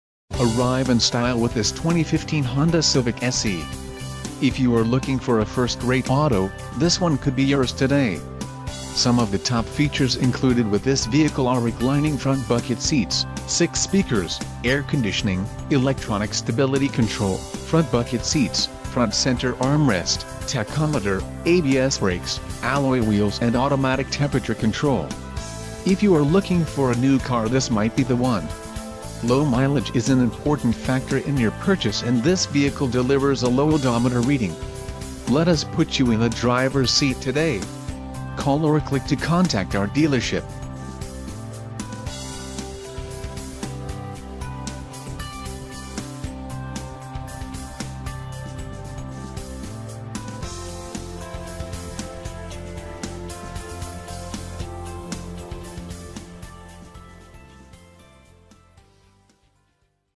2015 Honda Civic SE - Honda of Joliet - Joliet, Il 60435
2015 Honda Civic SE Condition: New Transmission: CVT Exterior Color: White Interior Color: Beige Doors: 4 Stock: J50359 Cylinders: 4